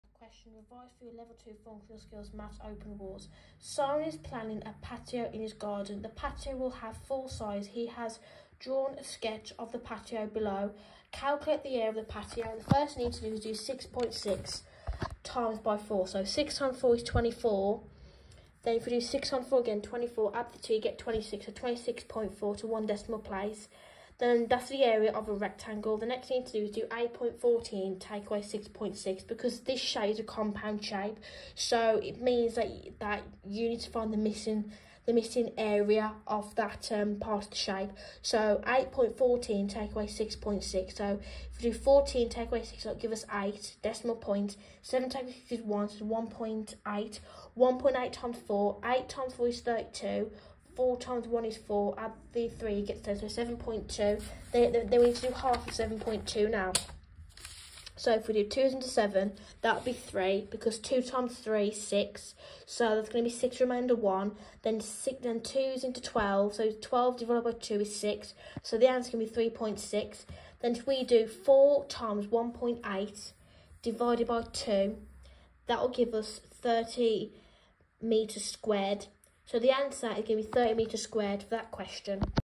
Area & Perimeter Exam Questions. Revise For Your Level 2 Functional Skills Maths Exam
Perimeter is distance around the shape the formula of a square is to add all the sides because sides on a Square are all even